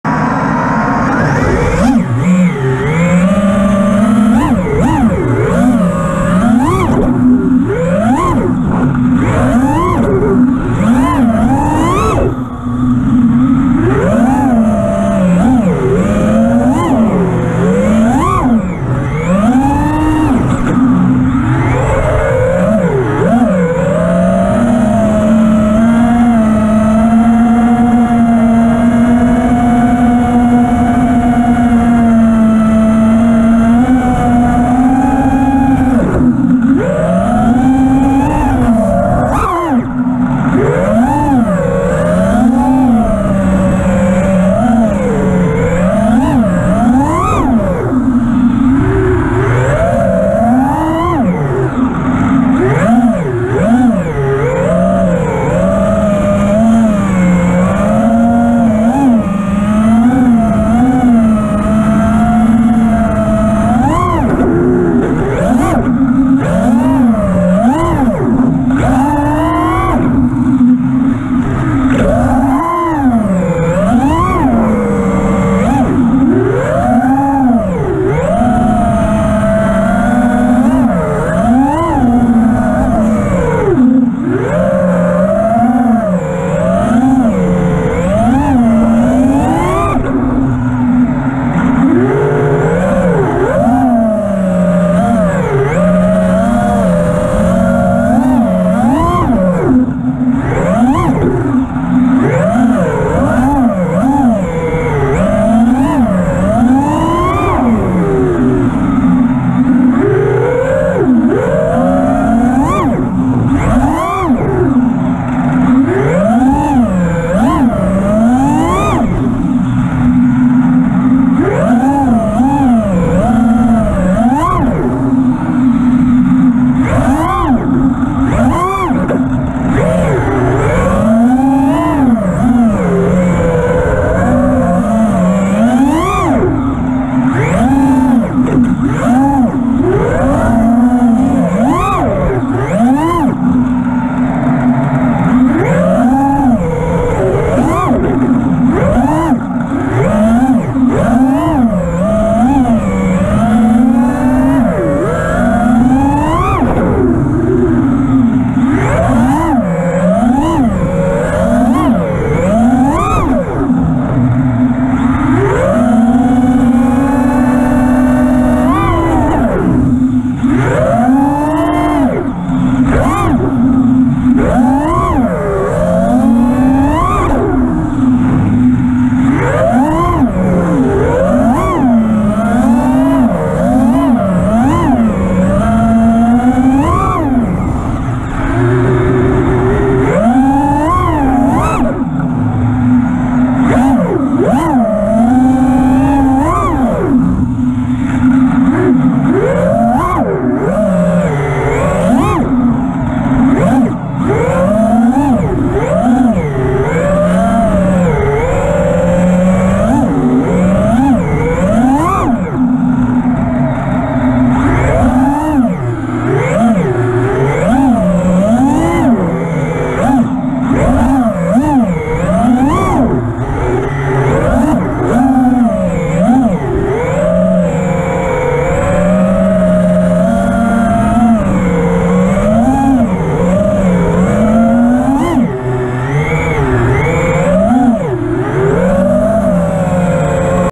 Swapped Fettec 45 Amp ESC for a 25 Amp Kiss ESC
Its surprising how big of a difference there is between ESC's crazy stuff